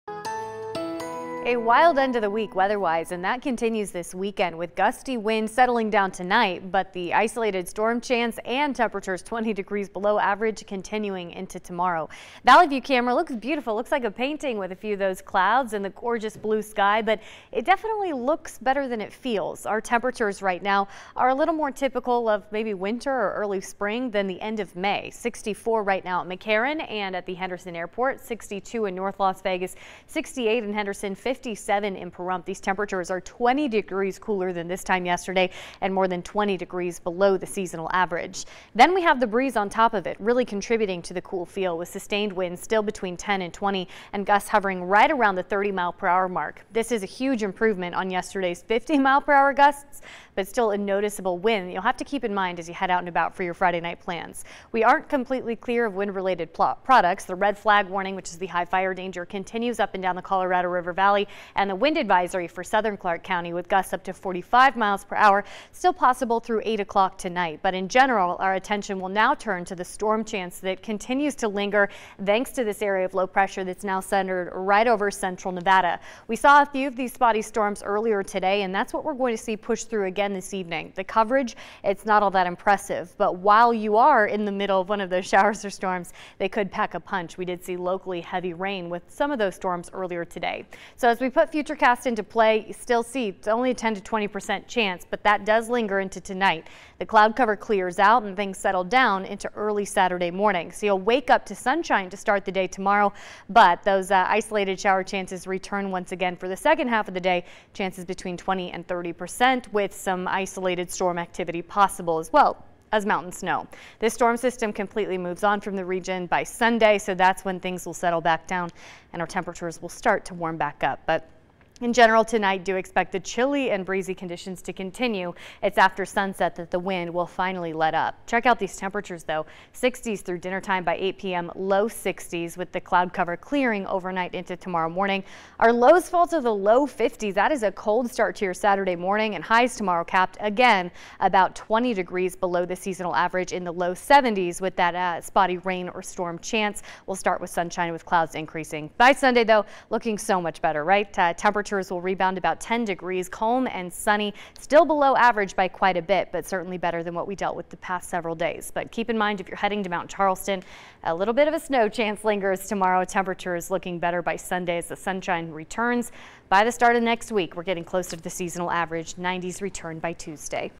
13 First Alert Las Vegas evening forecast | May 21, 2021
The May 21, 2021 evening weather forecast for Las Vegas.